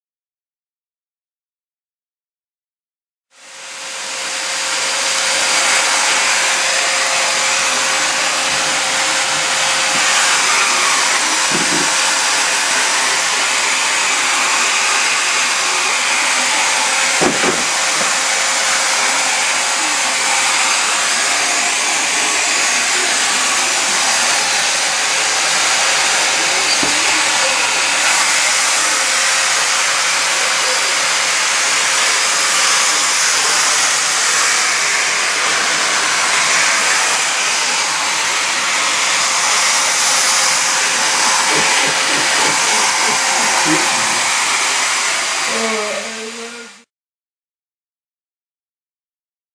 Hoovering.
Nancy ate a mince pie and got crumbs all over the floor...